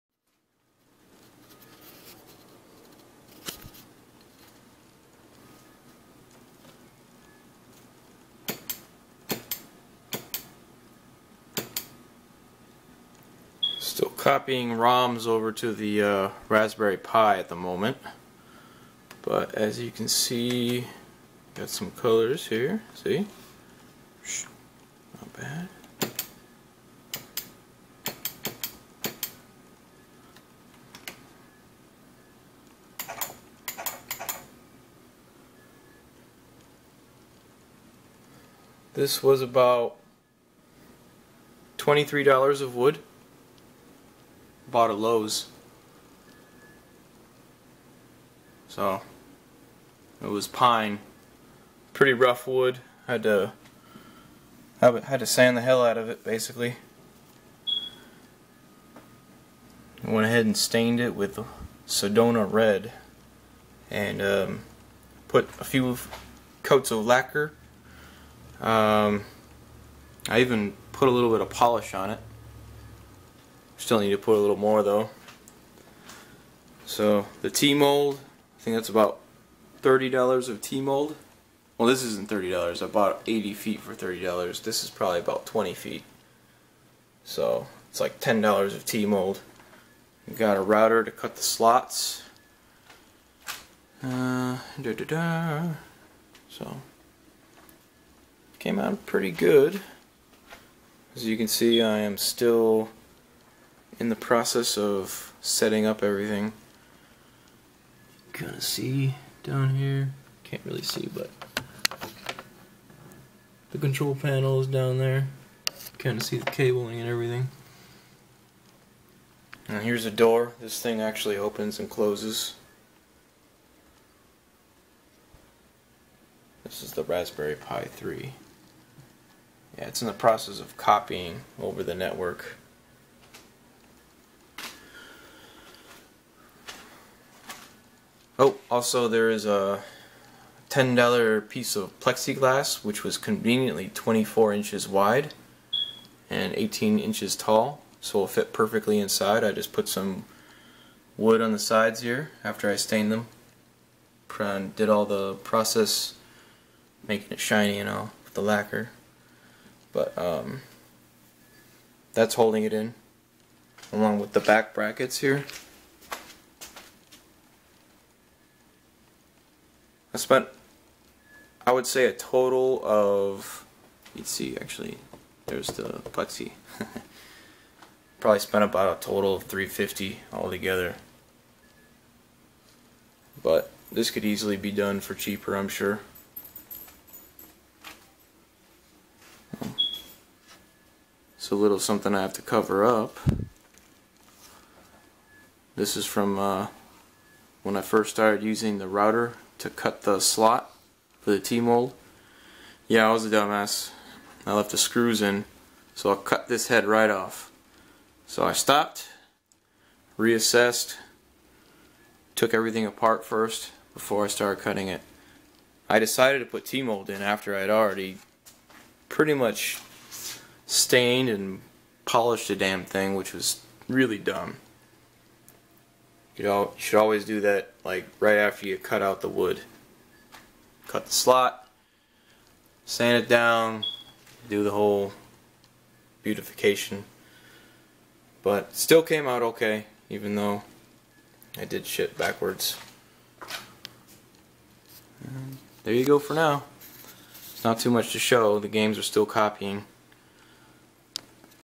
Custom built Arcade Bartop running Raspberry Pi 3
This is a quick (3 day) project involving the complete fabrication (cut out wood, sanding, staining, etc) of an arcade bartop (tabletop) machine running the Raspberry Pi 3. I used $23 of pine wood, a joystick/button kit from amazon ($76), $85 new monitor @ 22", sanded, stained in sedona red, laquer applied, polished, added $10 plexiglass, t-molding ($5), cropped, and used a power strip for all the power connections (2) for ease on/off. This was a quick fun project I gave to a buddy. I went about $350 in for all parts; however, this could have been much cheaper if you used all used equipment.